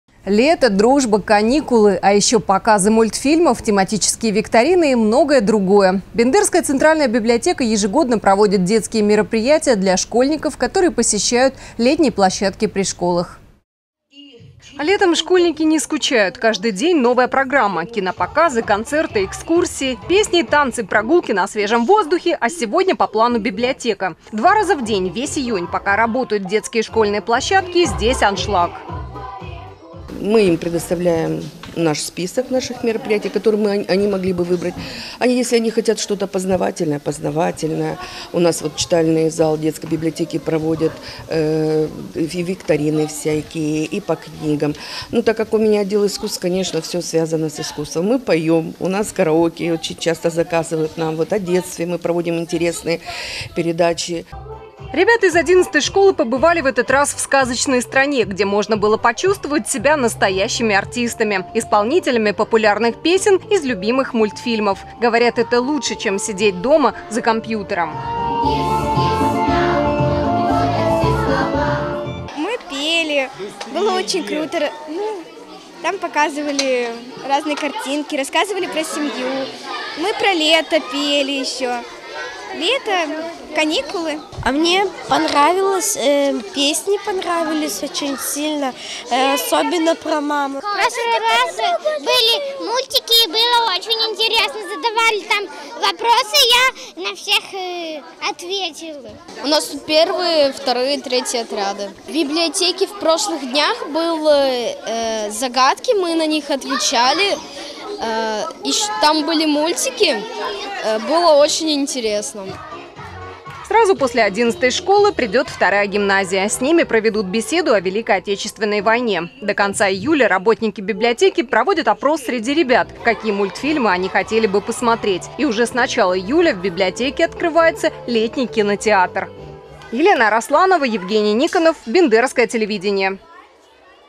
Лето, дружба, каникулы
Показы мультфильмов, тематические викторины и многое другое. Бендерская центральная библиотека ежегодно проводит детские мероприятия для школьников, которые посещают летние площадки при школах.